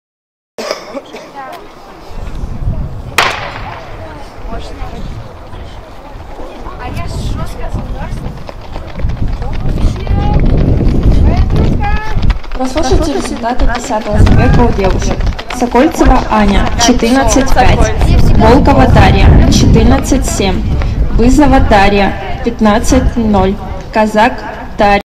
2019 Чемп ИО | 100м М Q2
Открытый чемпионат Иркутской области  по легкой атлетике, посвященный Дню Победы